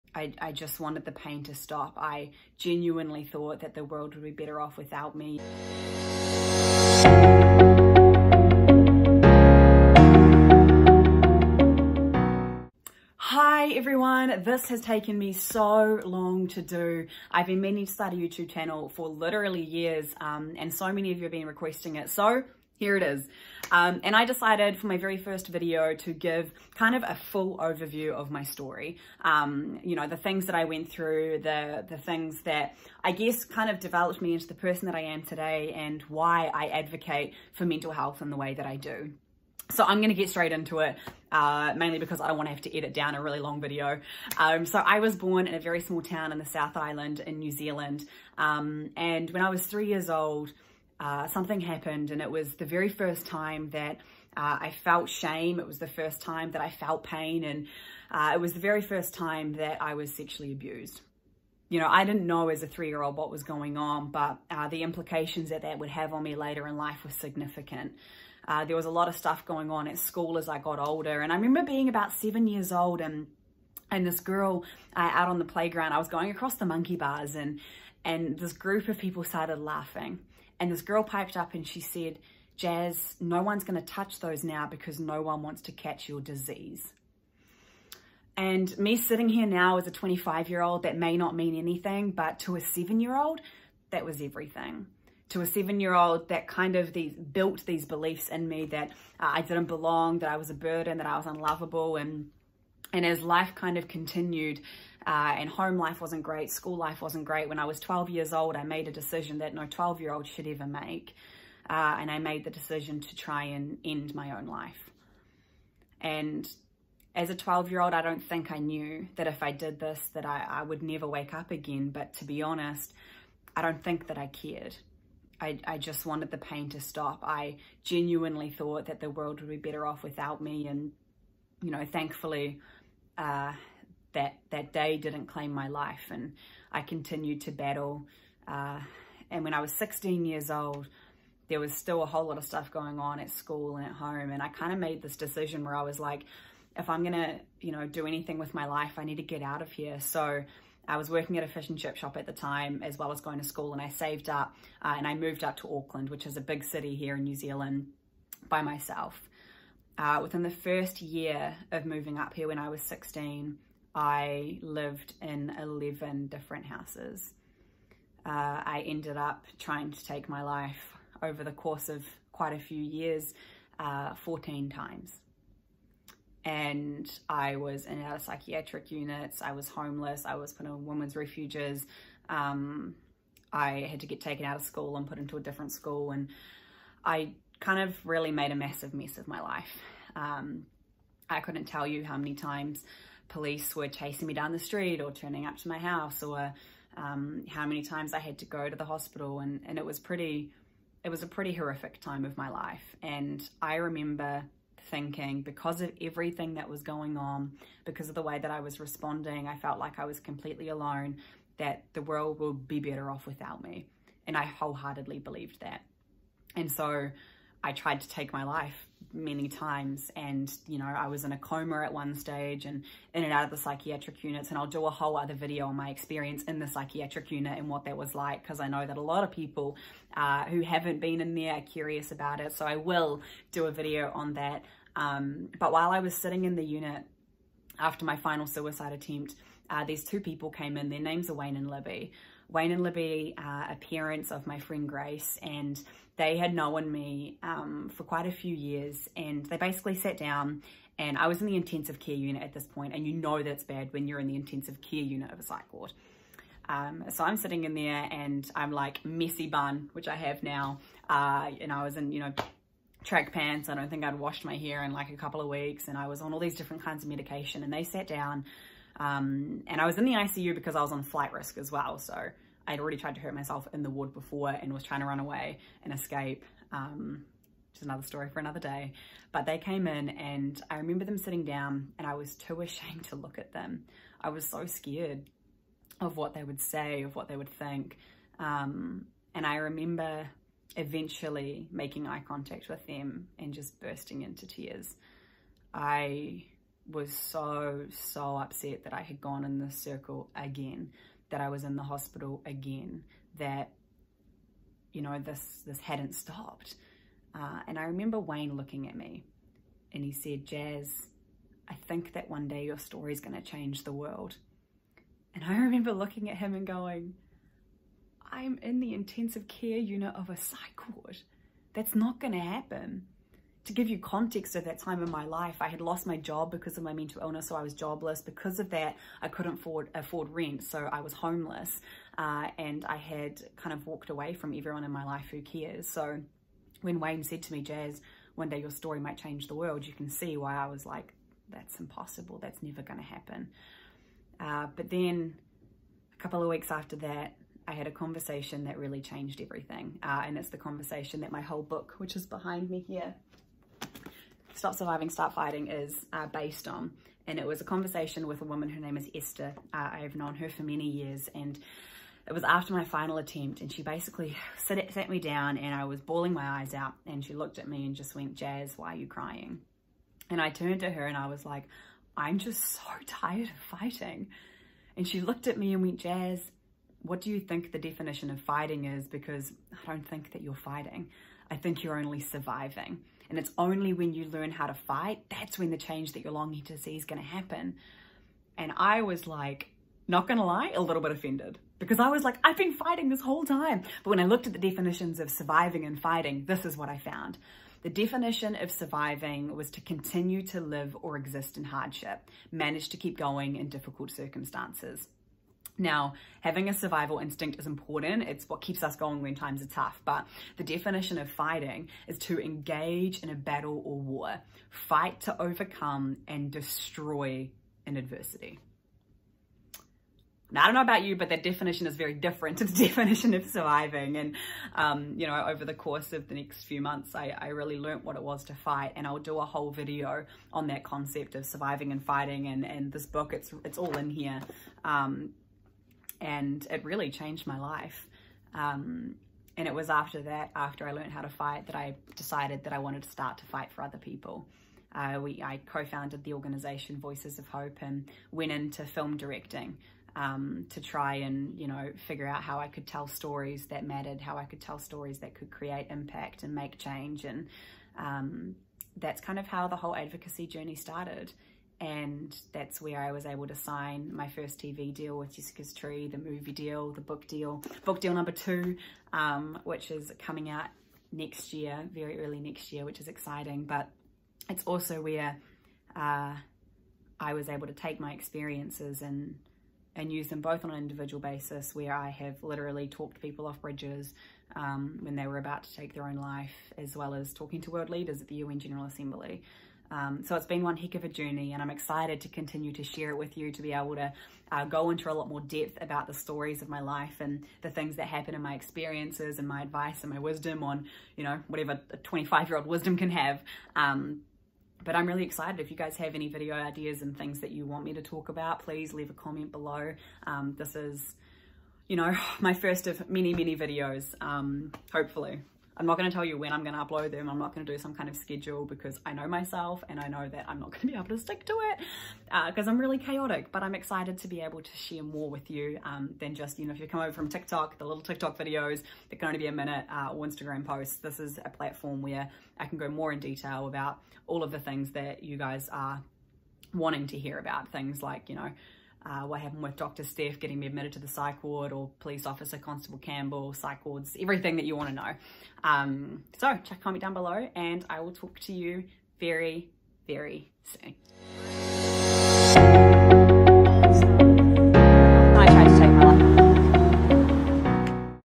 Suicide, Mental Illness & Hope.. My story
TRIGGER WARNING: Suicide, Mental Illness, Psych wards.
This is my first ever YouTube video, so I thought that I would give you an overview of my story. Let me know what topics or stories you want covered in the comments below!

If you need immediate help people contact your local police.